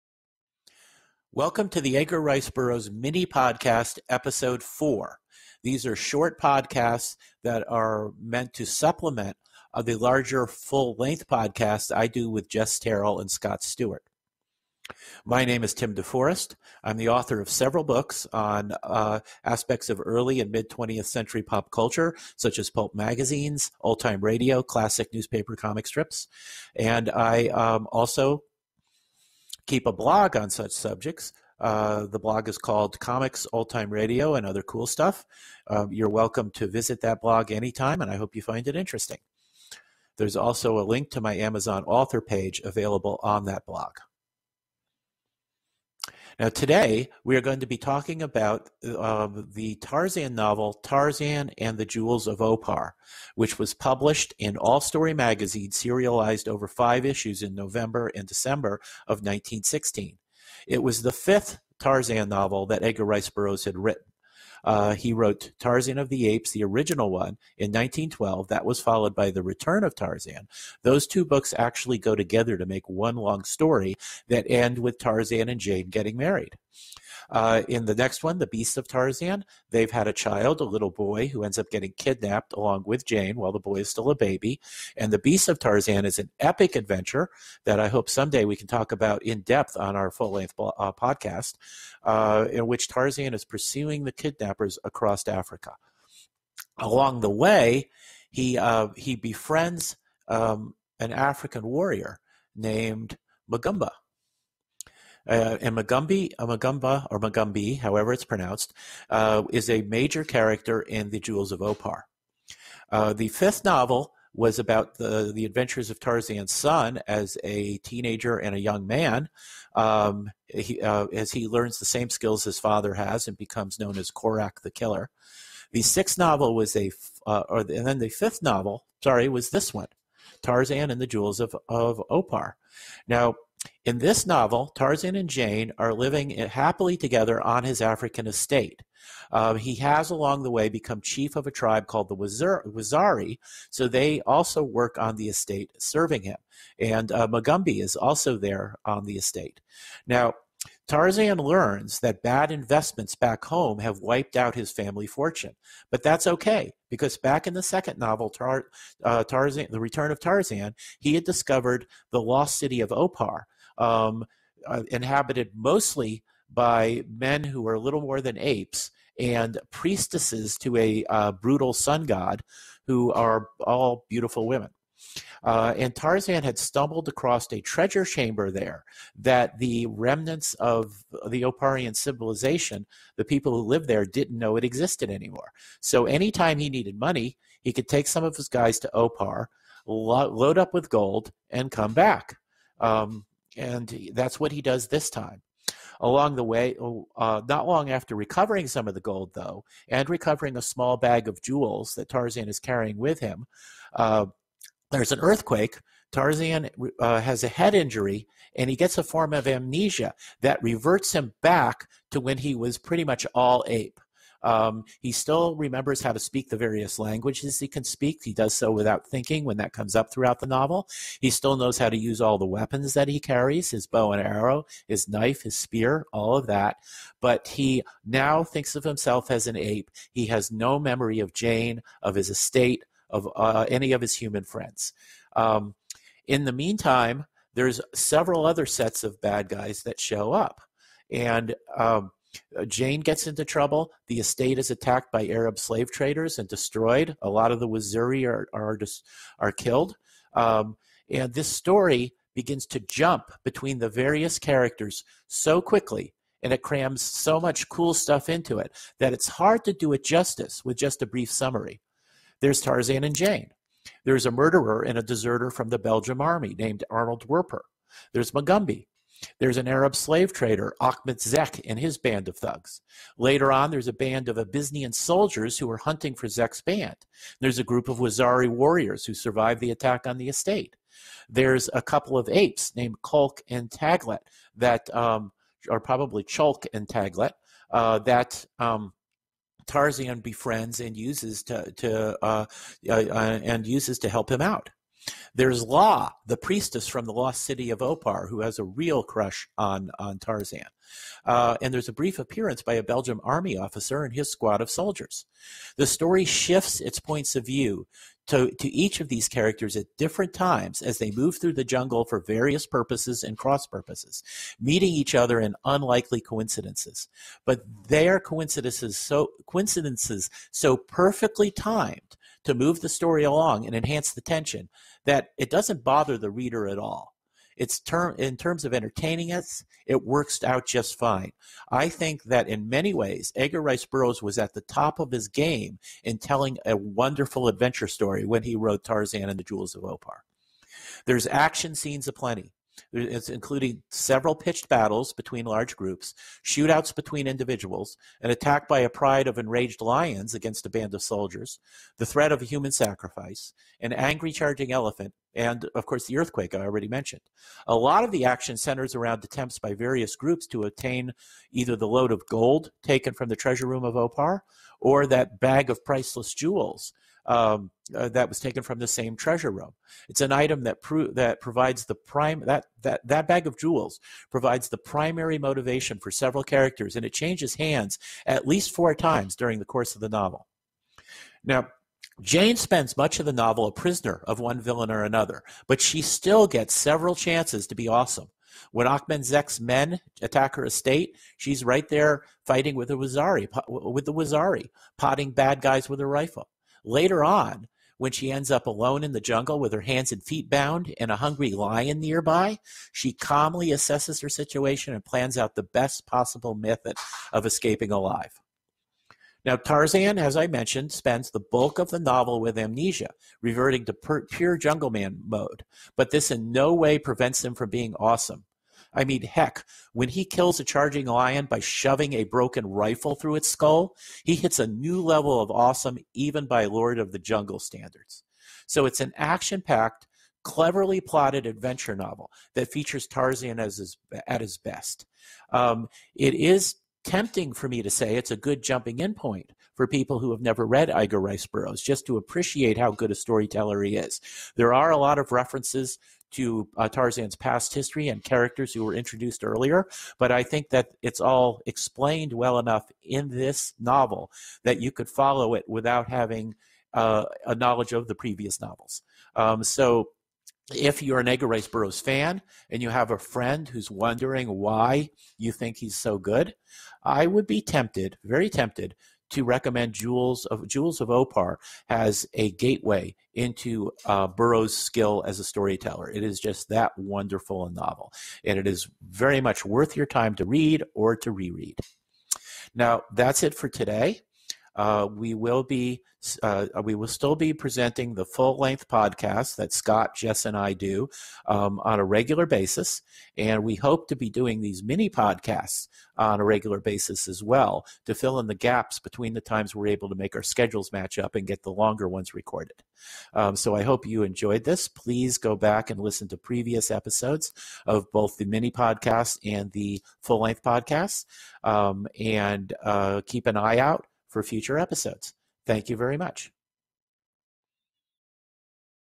Edgar Rice Burroughs Mini-Podcast #4: The Jewels of Opar
Tim DeForest geeks out over the 1916 novel "Tarzan and the Jewels of Opar."